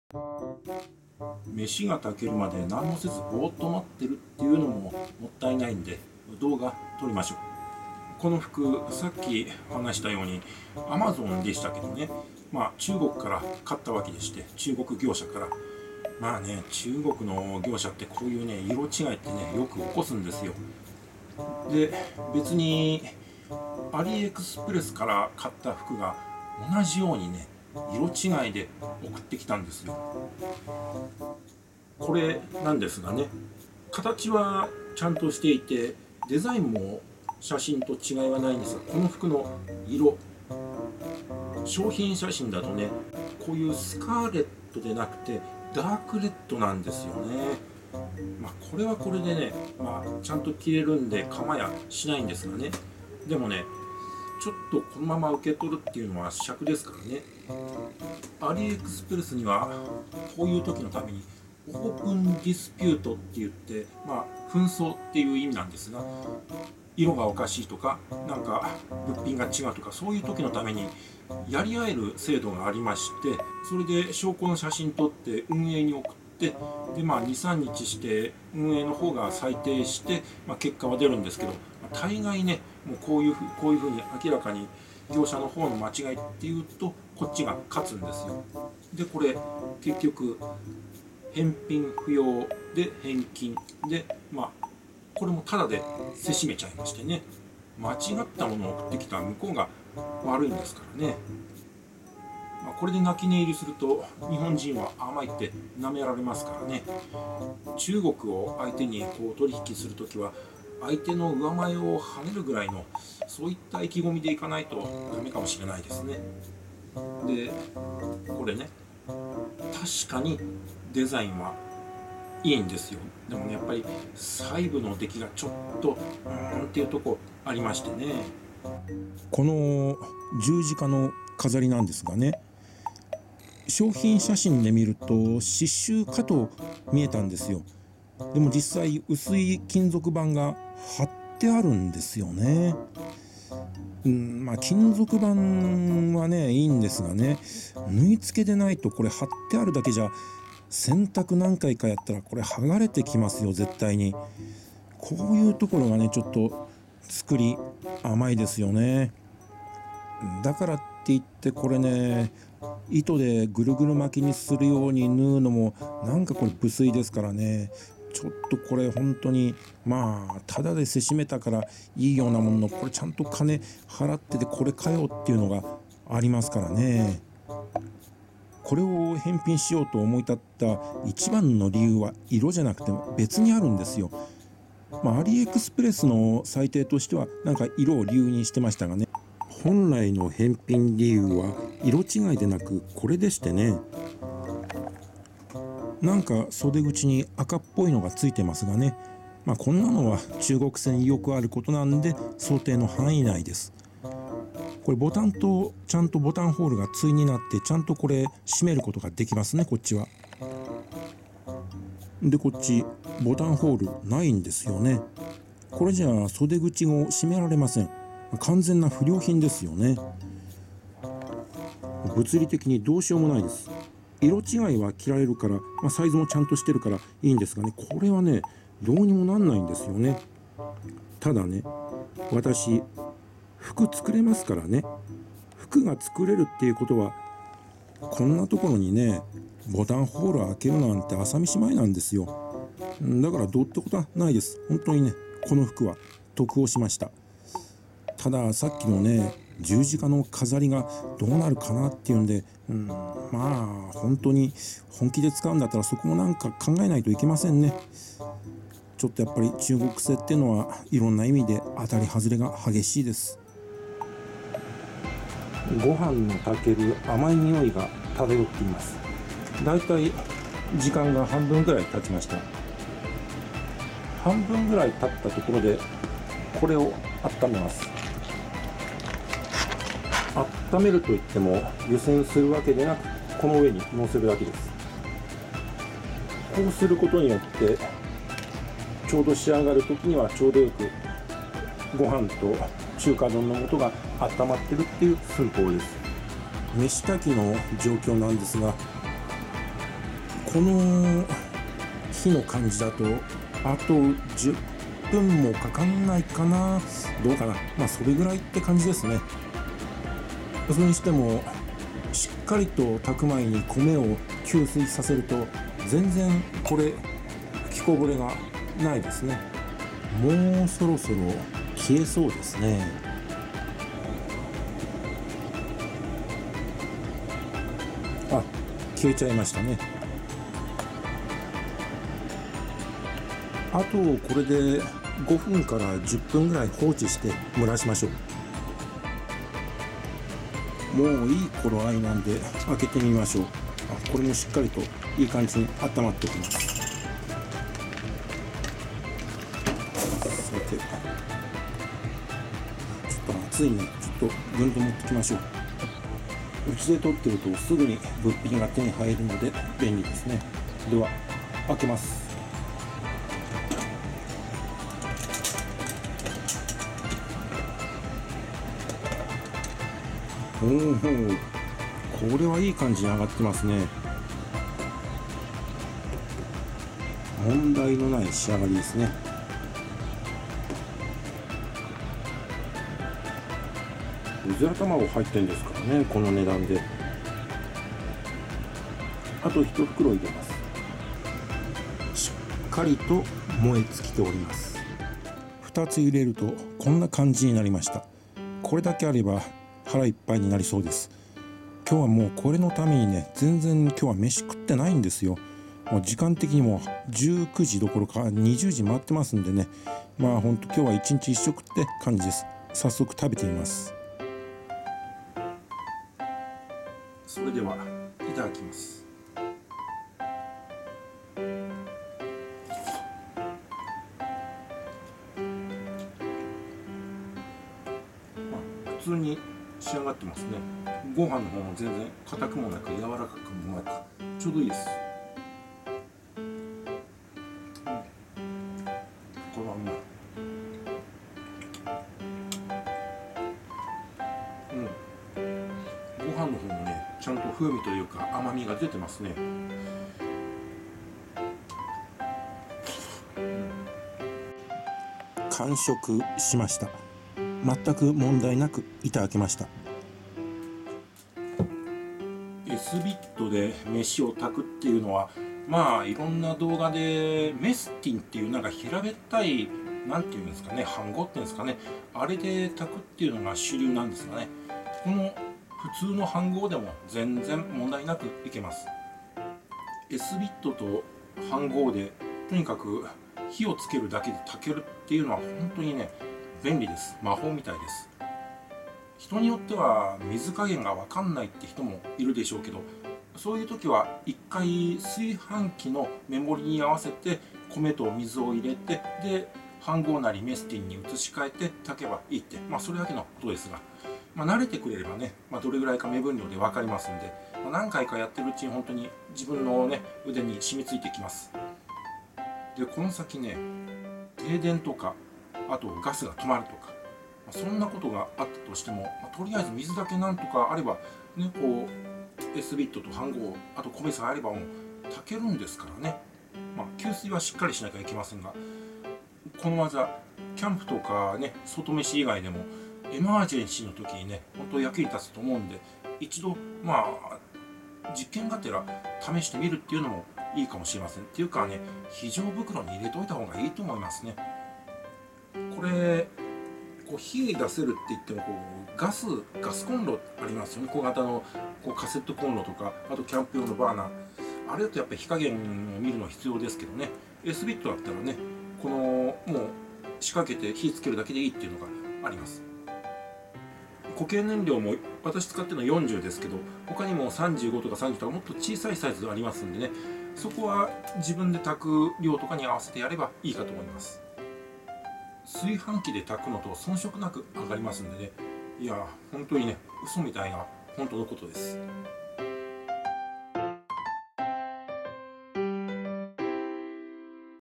第59番-2 タダでせしめる・自動炊飯中華丼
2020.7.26に撮った動画です。
中国通販は色々と雑なので逆に得したりもします。
そうこうしてるうちに自動炊飯が終わったようです。

今まで撮った自撮り写真が沢山あるので是非ご覧ください。